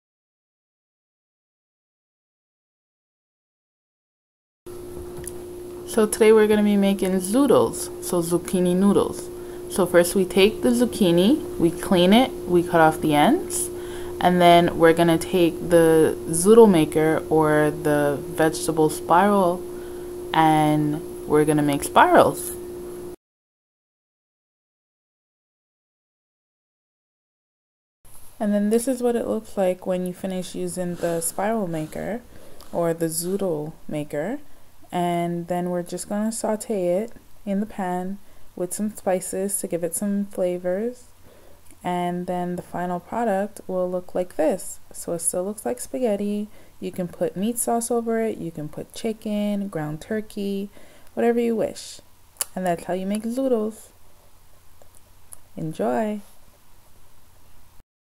How to make Zoodles (Zucchini Noodles)
Here's a easy way to make Zoodles (zucchini noodles).

Ingredients:
3-4 zucchinis
vegetable spiral tool

Directions:
1. Clean and wash the outside of the zucchini
2. Cut off the end of the zucchini (you can decide to only cut one end but it's just simpler to cut both).
3. Spiral the zucchini
4. Sauté in a pan for 5 mins (stirring often)
5. Serve hot
6. ENJOY!!!

This serves approximately 3 servings.

Want more cooking tips, recipes and how to's?